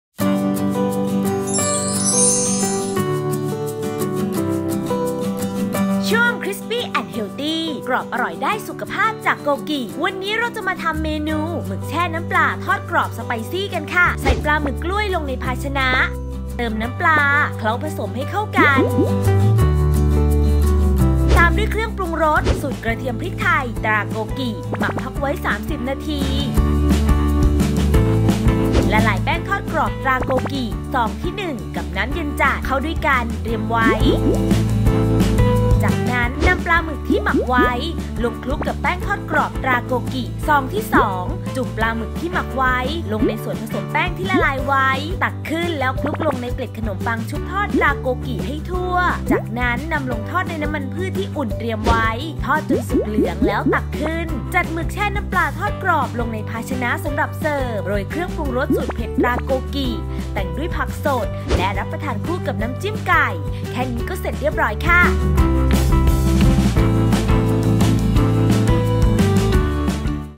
หมึกแช่น้ำปลาทอดกรอบสไปรท์ซี่ แจกสูตรวิธีทำแบบง่ายๆ โดย "อ.ยิ่งศักดิ์" & โกกิ
ผลิตภัณฑ์ : แป้งทอดกรอบ ฮอทแอนด์สไปซี่ 2 in1 กระเทียมพริกไทย ตราโกกิ Gogi brand tempura flour with 2 flavours Hot & Spicy Garlic Pepper

เกล็ดขนมปังชุบทอด ตราโกกิ : Gogi brand bread crumbs

ปลาหมึกกล้วยหั่นเป็นวง     250 กรัม
เครื่องปรุงรสสูตรกระเทียมพริกไทย ตราโกกิ  1 ซอง
แป้งทอดกรอบ ตราโกกิ (ซองที่ 1)   1  ซอง
น้ำเย็นจัด      140 มิลลิลิตร
แป้งทอดกรอบ ตราโกกิ (ซองที่ 2)   1  ซอง
เกล็ดขนมปังชุบทอด ตราโกกิ   1  ซอง

เครื่องปรุงรสสูตรเผ็ด ตราโกกิ ปริมาณตามชอบ
ผักสด และน้ำจิ้มไก่ สำหรับรับประทานคู่

1. ใส่ปลาหมึกกล้วยลงในภาชนะเติมน้ำปลาเคล้าผสมให้เข้ากันตามด้วย เครื่องปรุงรสสูตรกระเทียมพริกไทย ตราโกกิ หมักพักไว้ 30 นาที

2. ละลาย แป้งทอดกรอบ ตราโกกิ (ซองที่ 1) กับน้ำเย็นจัด เข้าด้วยกัน เตรียมไว้

3. นำปลาหมึกที่หมักไว้ลงคลุกกับ แป้งทอดกรอบ ตราโกกิ (ซองที่ 2) จนทั่วดี จุ่มปลาหมึกที่หมักไว้ลงในส่วนผสมแป้งที่ละลายไว้ ตักขึ้นคลุกลงใน เกล็ดขนมปังชุบทอด ตราโกกิ  ให้ทั่ว

4. จากนั้นนำลงทอดในน้ำมันพืชที่อุ่นเตรียมไว้ ใช้ไฟปานกลาง ทอดจนสุกเหลือง ตักขึ้น

5. จัดหมึกแช่น้ำปลาทอดกรอบลงกาชนะสำหรับเสิร์ฟ โรย เครื่องปรุงรสสูตรเผ็ด ตราโกกิ แต่งด้วยผักสดรับประทานคู่กับน้ำจิ้มไก่

Line @yingsakfood